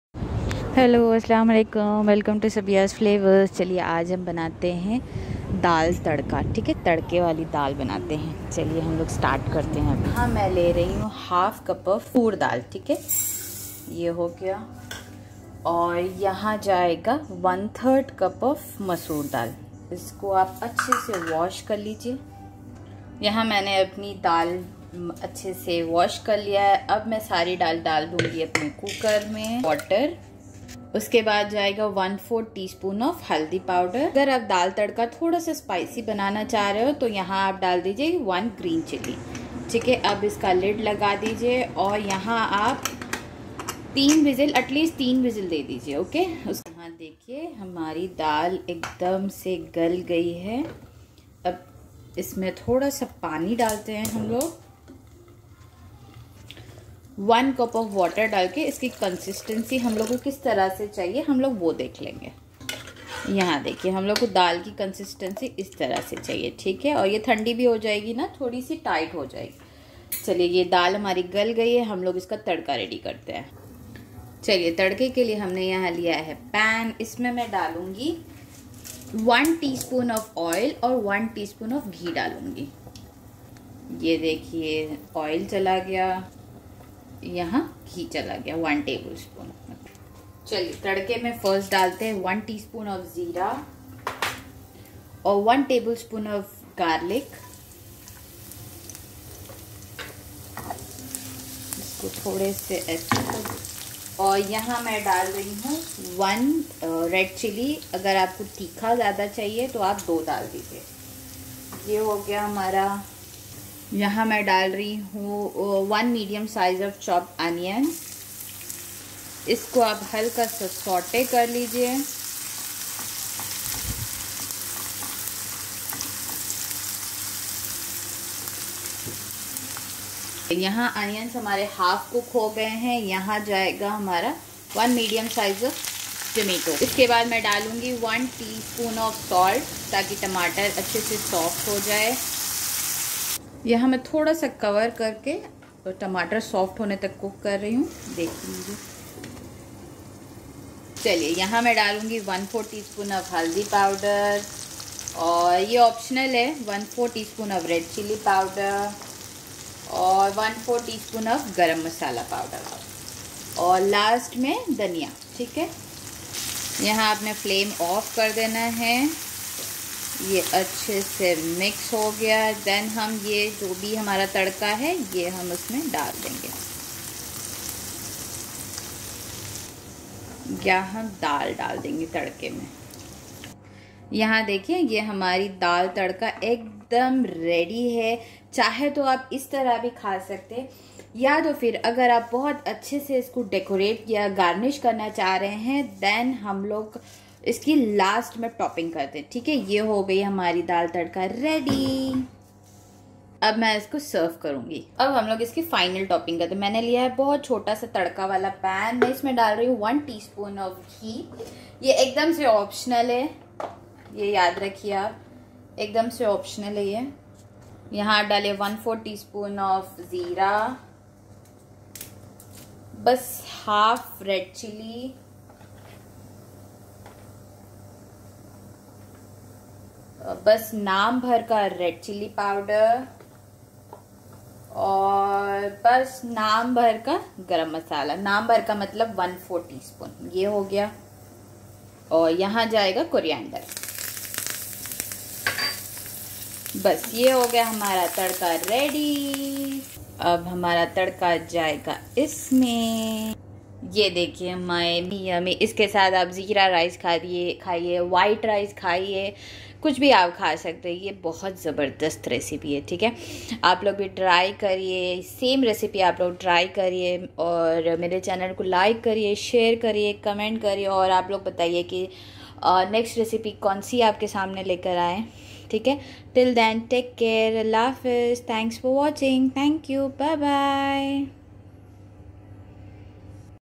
Dal Tadka | dal tadka kaisa banate hain/restaurant Style Dal Fry | Dal fry at home |दाल तड़का/uae
Homestyle recipe for hearty and delicious Dal Tadka, also known as Dal Fry. In this satisfying Indian comfort food, split yellow pigeon peas (toor dal) are cooked with aromatics and mild spices, then finished with a spiced tempering called 'tadka'. Enjoy this easy Instant pot recipe with the option to make basmati rice in the same pot.

Dal Tadka Ingredients

Toor dal ½cup
Masoor dal ⅓cup
Harimirch 1
Haldi powder ¼tsp
Mirchi powder ½tsp
Salt ½tsp

Tadka--
Oil& ghee1-1 tbsp
Zeera 1tsp
Onion ½cup
Tomato ½cup

Please try the recipe !!!

More recipes to watch

Thanks for 👀
Happy cooking 🍳😋